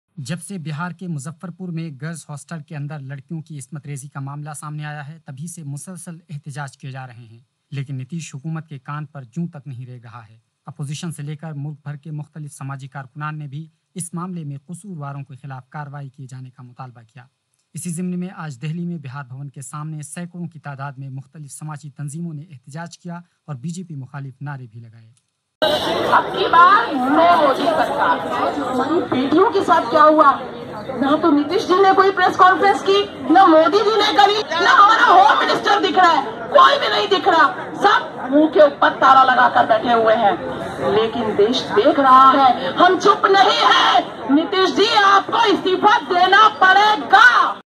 دہلی اب کی بار نو مودی سرکار: مظاہرین
جب سے بہار کے مظفر پور میں گرلس ہاسٹل کے اندر لڑکیوں کی عصمت ریزی کا معاملہ سامنے آیا ہے تبھی سے مسلسل احتجاج کیے جا رہے ہیں لیکن نتیش حکومت سرد مہری غالب ہے۔